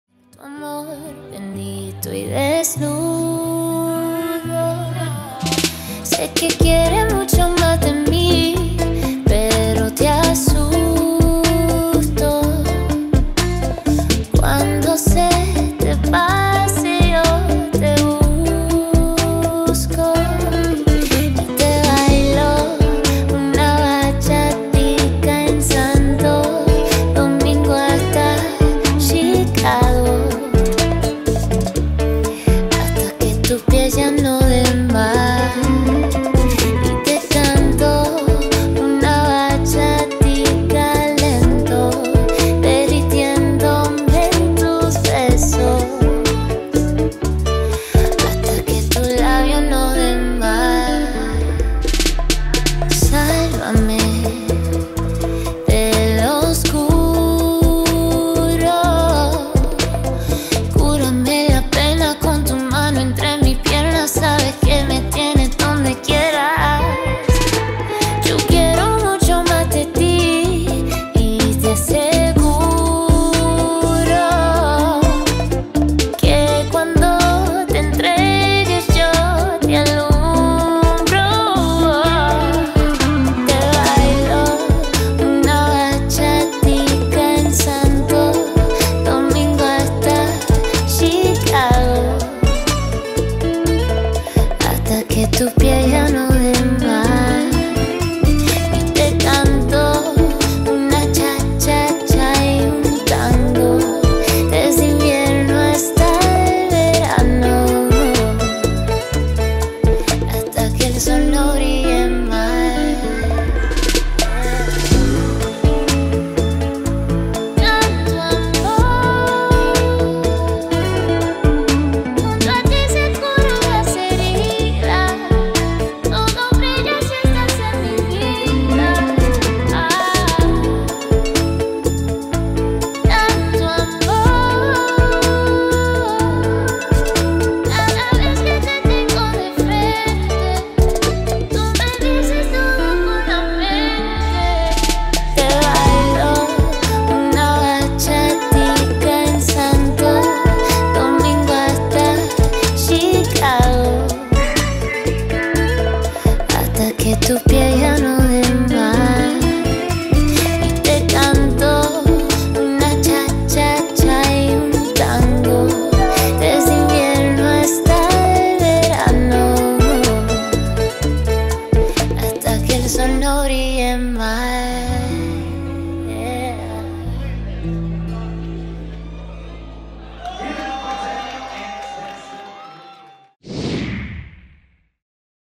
Daniel & Desiree - Social dance [Bachatica] | Latin Beach Fest DR 2021 (Punta Cana)
Social Dancing

October 14-18th
Latin Beach Fest 2021 | Punta Cana, DR